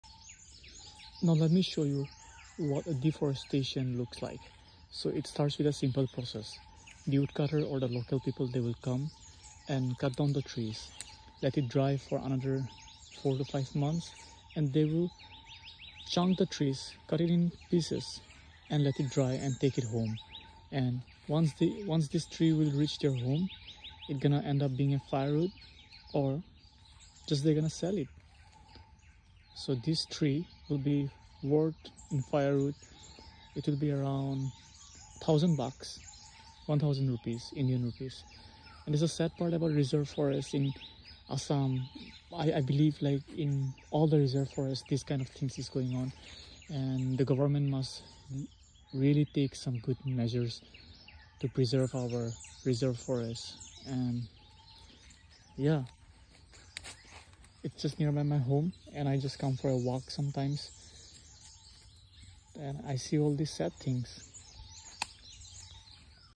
Deforestation (the process)
This is happening in 2023 in a place called Golaghat, Assam , India.